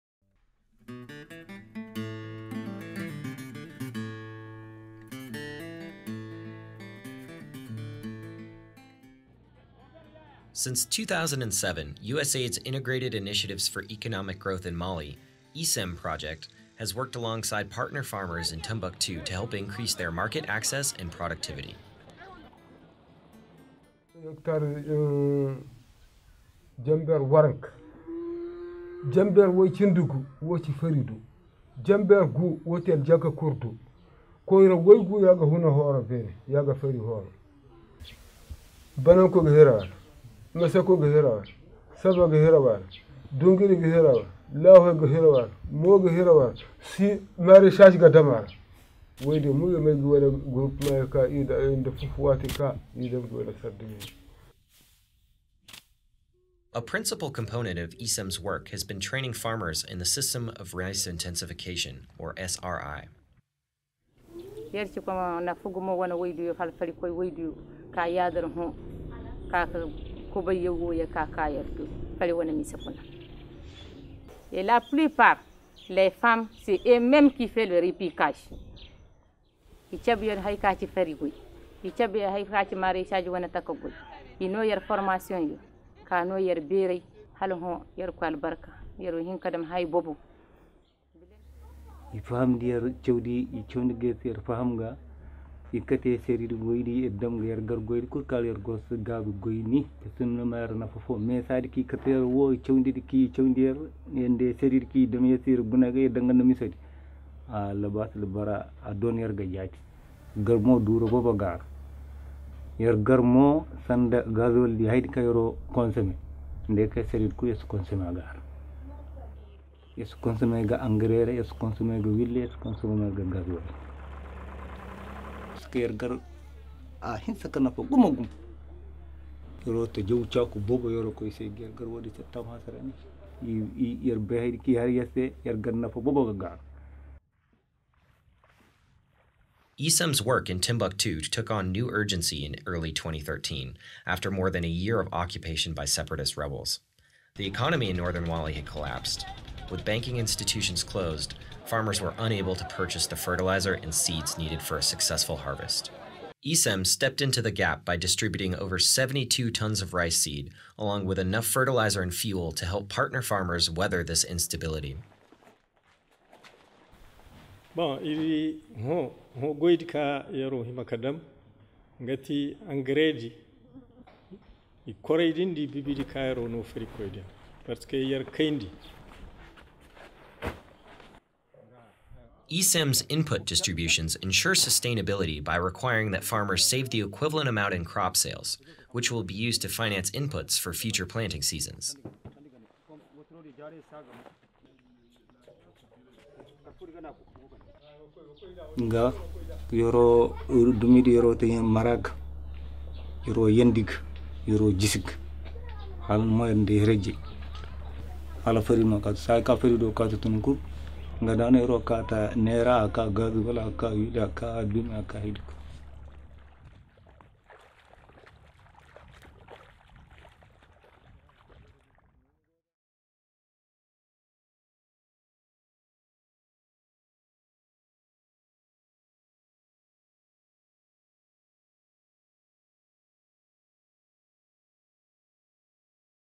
Reinforcing Food Security in Timbuktu
IICEM  is USAID/Mali's cornerstone economic growth program under the U.S. government's Feed the Future initiative.